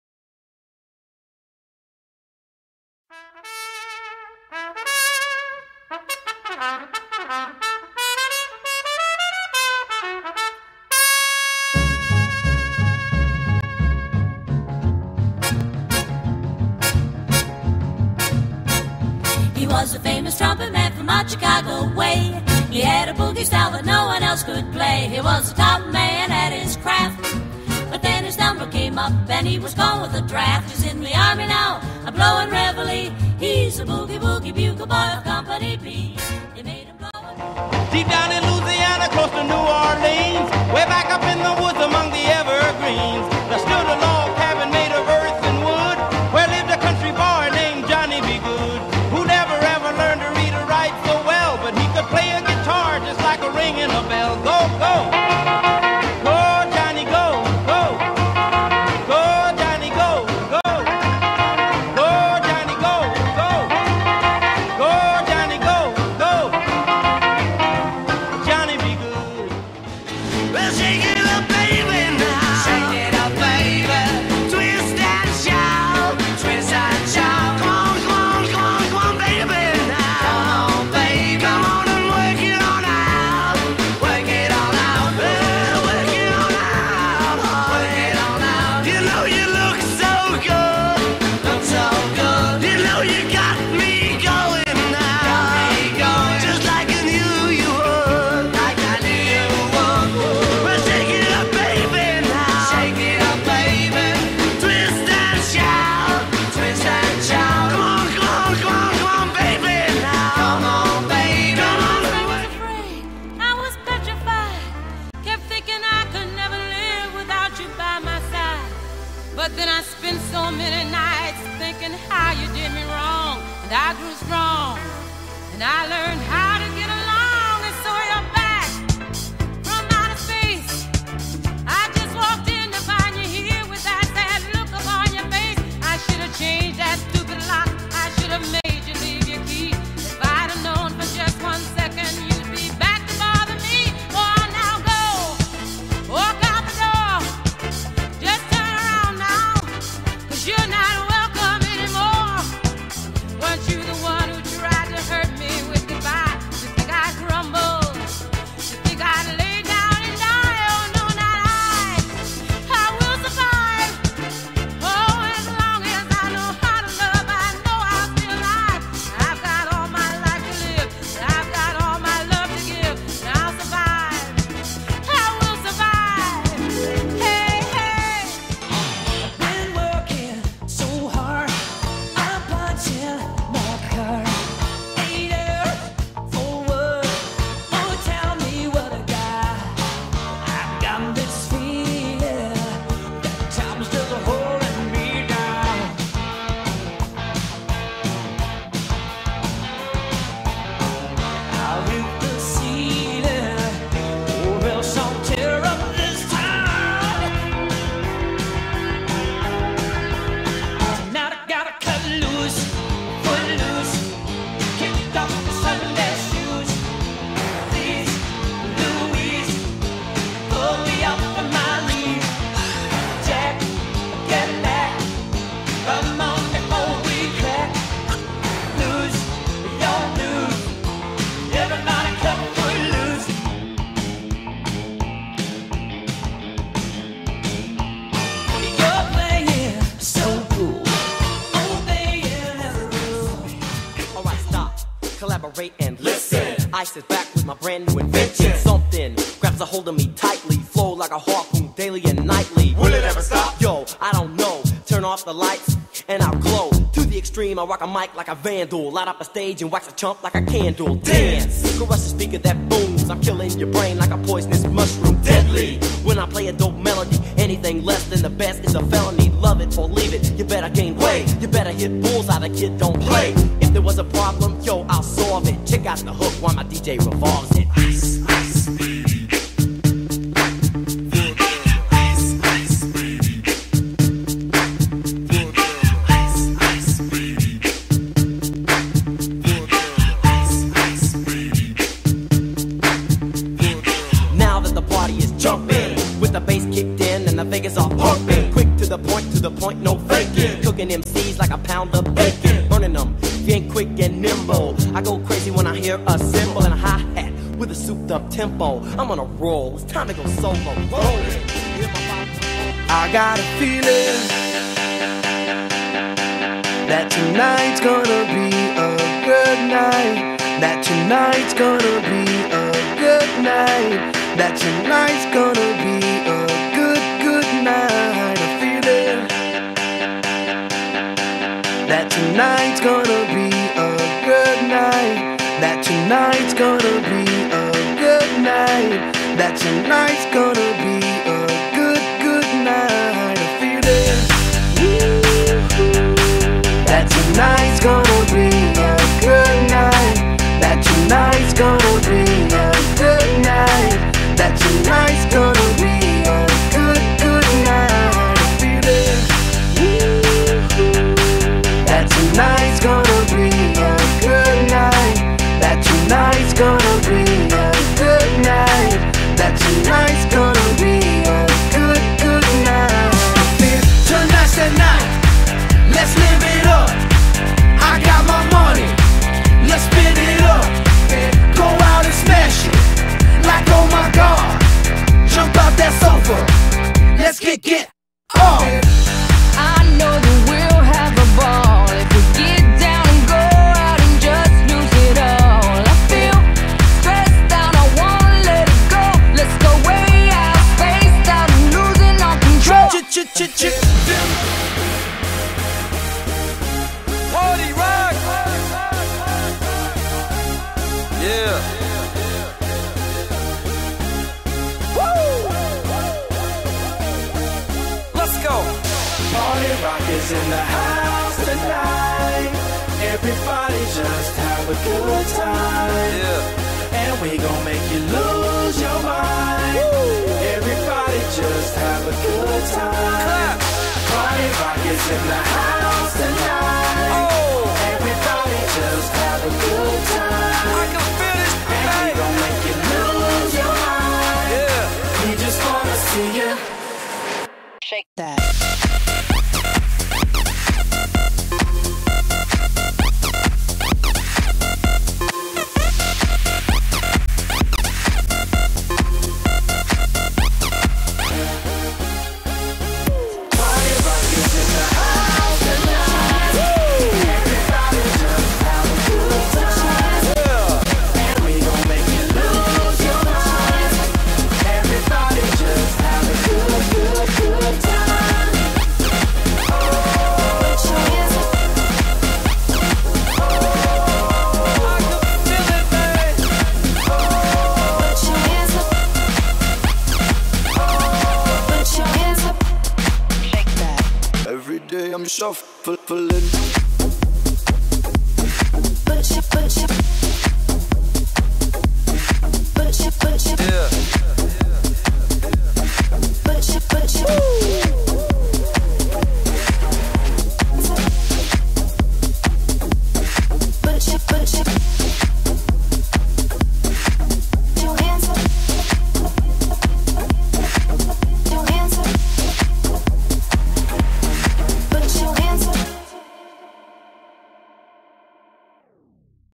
Walker Creek Elementary 2012 lib dub.mov
students and teachers travel from the 40s to today in this walk through the school.